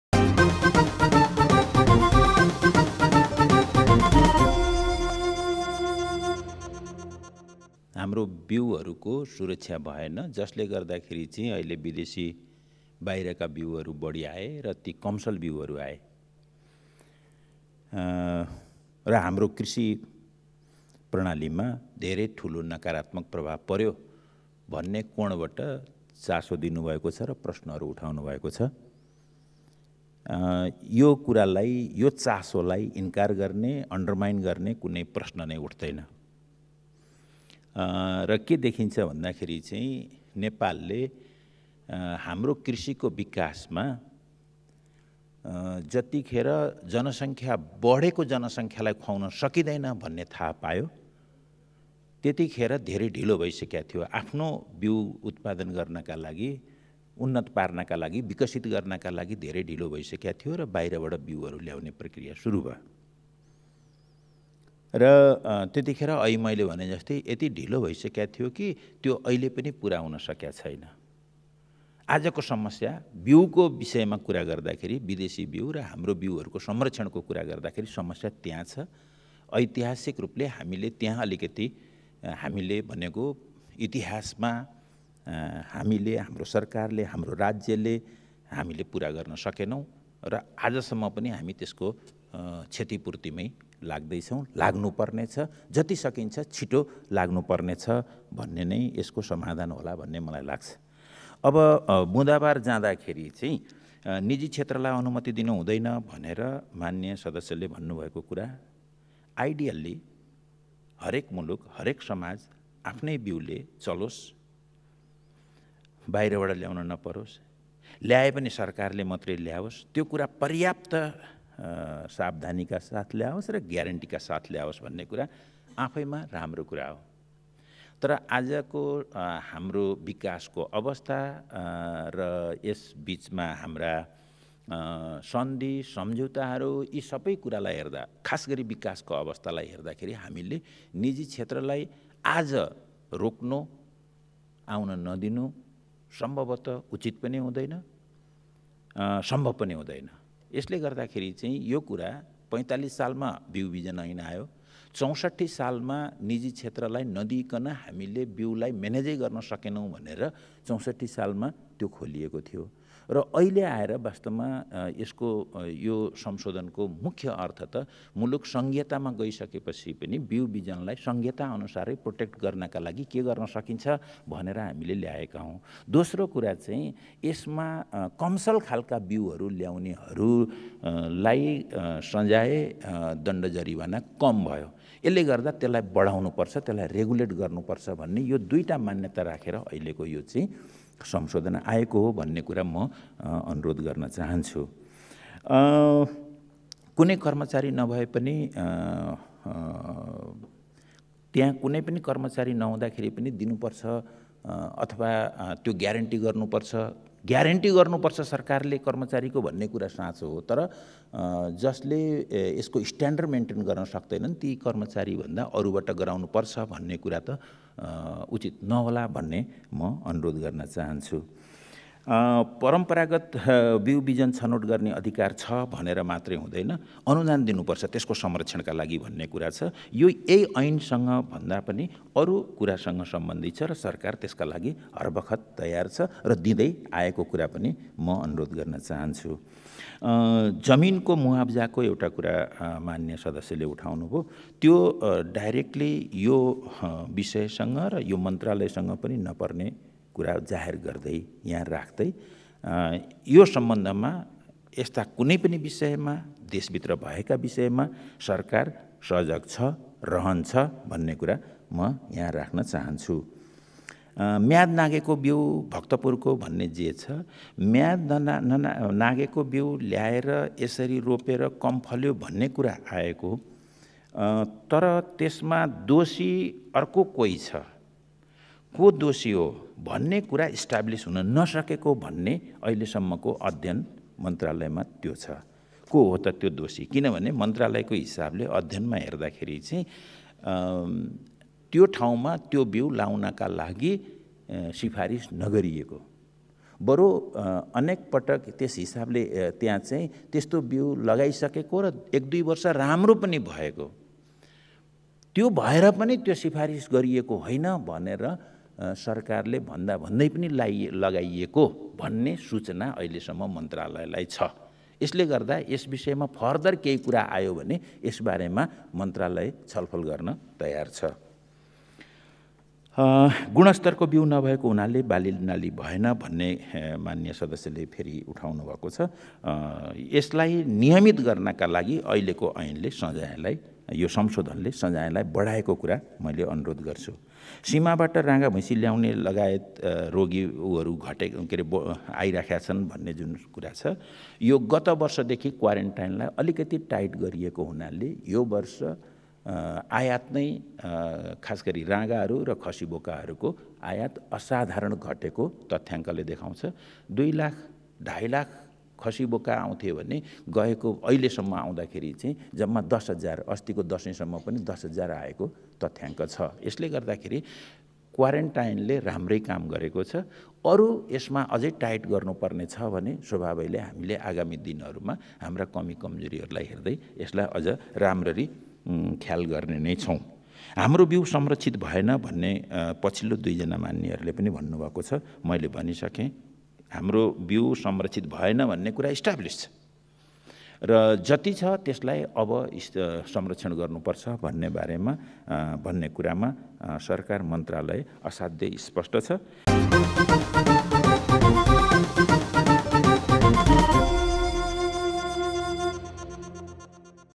कमसल बिहु बेचबिखन गर्नेलाई कडा सजाए गरिन्छ: घनश्याम भुसाल Ghanashyam Bhusal
Ghanashyam_Bhusal कमसल बिहु बेचबिखन गर्नेलाई कडा सजाए गरिन्छ : घनश्याम भुसाल